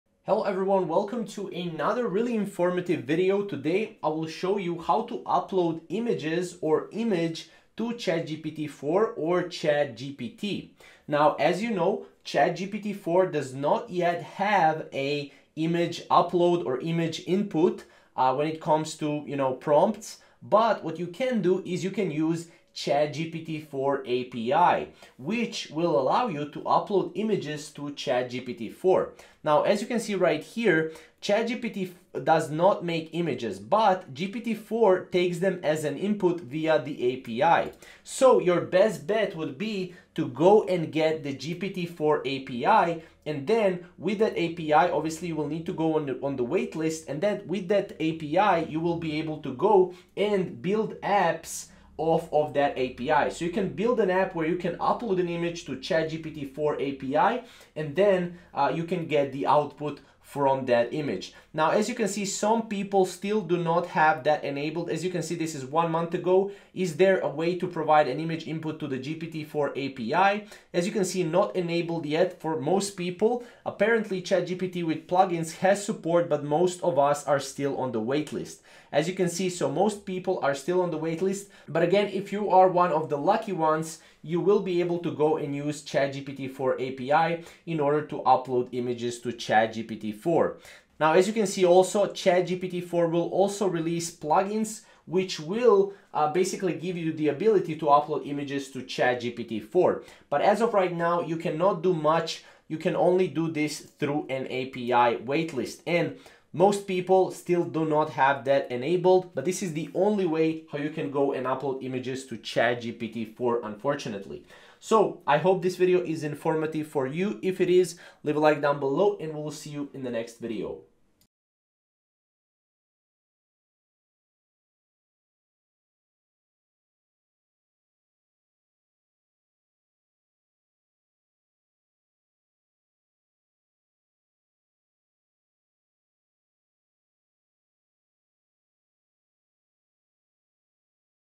How To Upload Image to GPT4/ChatGPT?
In this video, I will show you how to upload image to gpt4/chatgpt?